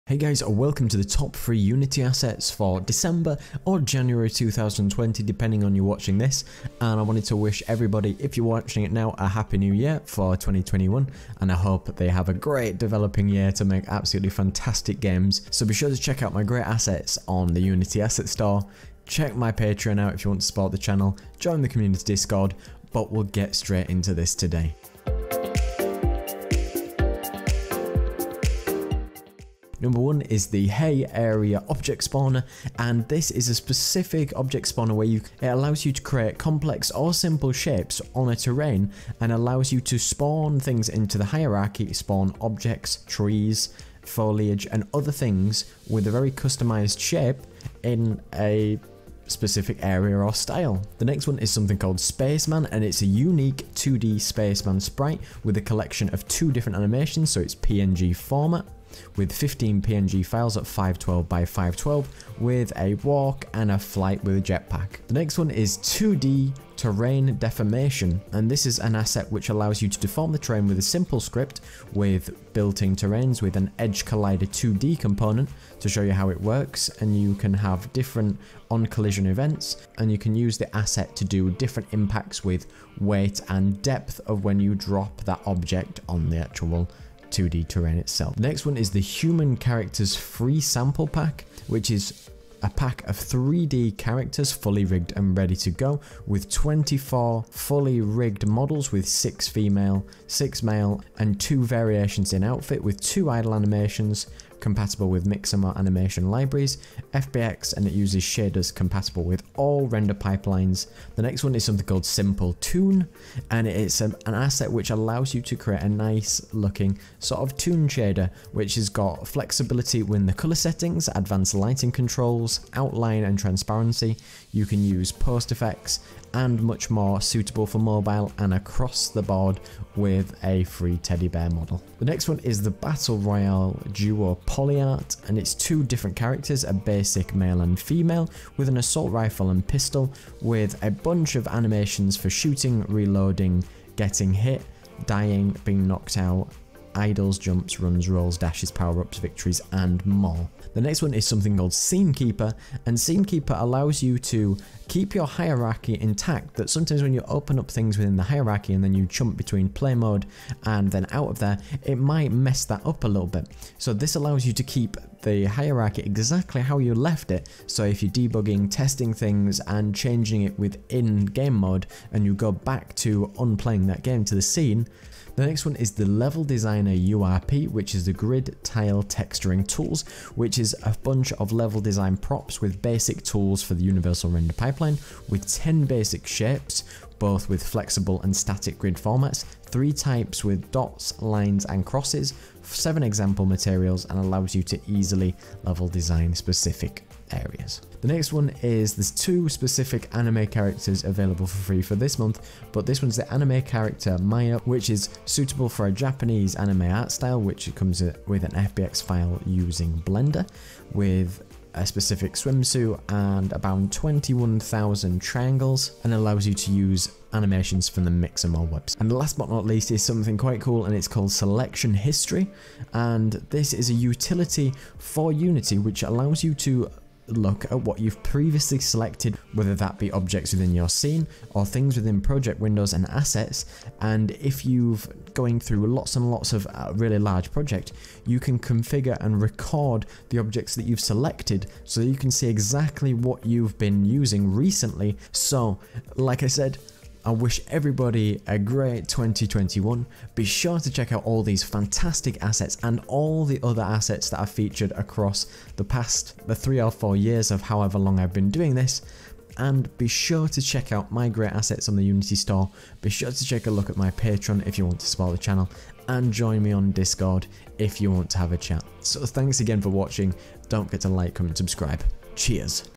FREE Unity Assets - January 2021
A selection of the best free assets for Unity, for December 2020 / January 2021. This includes Free models, Programming Assets, Materials, Textures, Particles, VFX and more!

1). Battle Royale Duo Polyart
2). Hey Area Object Spawner
5). Human Character (FREE Sample Pack)
8). Level Designer (URP) Grid Tile Texturing Tools